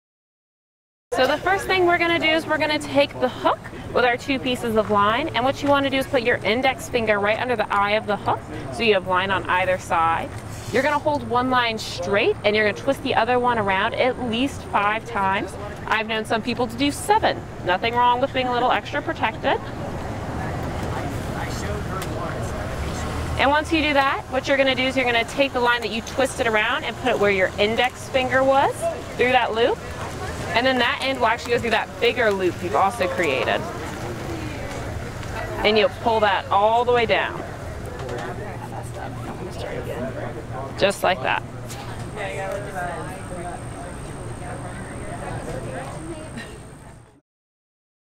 How to tie an improved clinch knot
FWC staff Hannah Healey goes over how to tie an improved clinch knot.